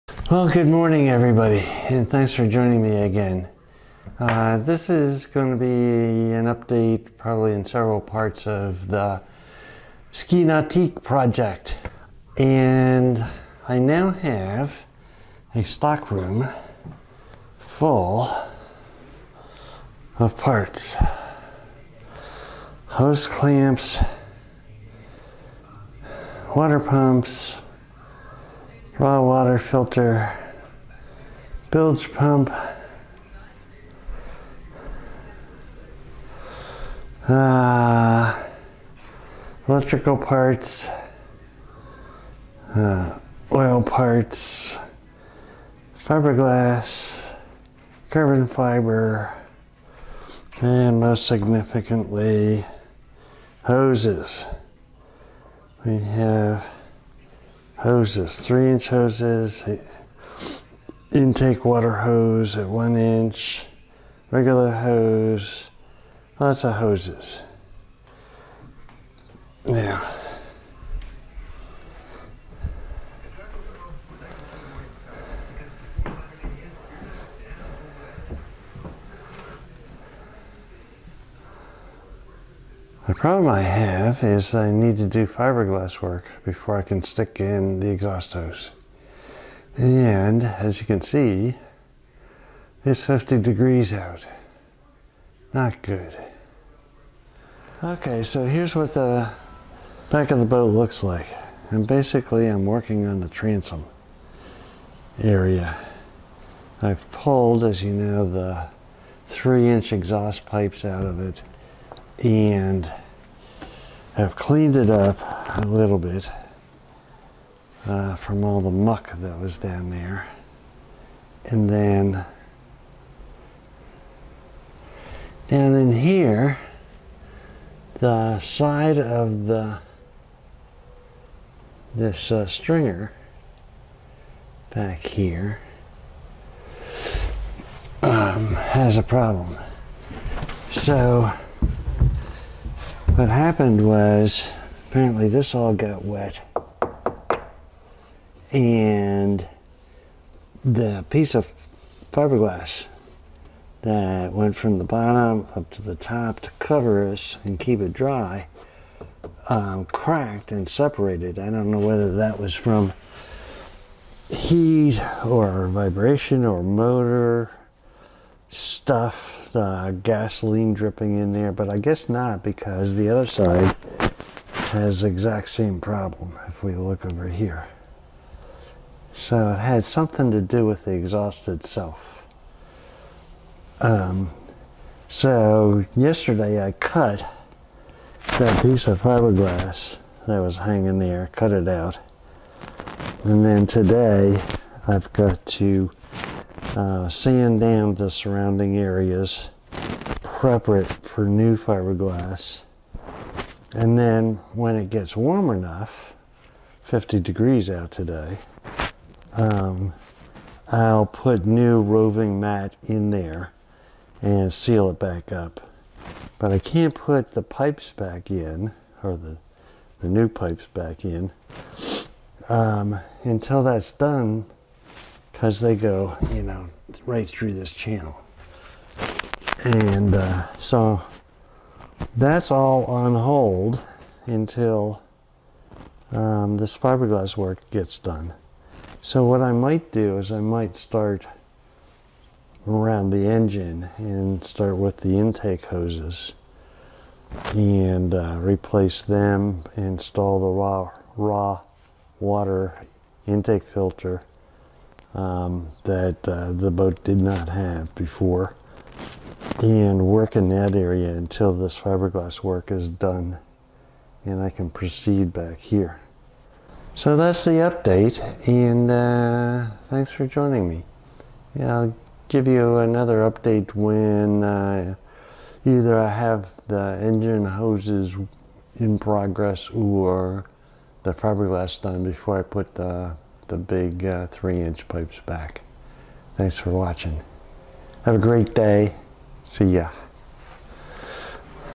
Ski Nautique Project Part 2
I have an inventory of parts now but I need to fiberglass both sides of the exhaust stringers.  The temperature outside is only 50 degrees (F) so it will have to wait.  Meanwhile I will start working on the intake hoses.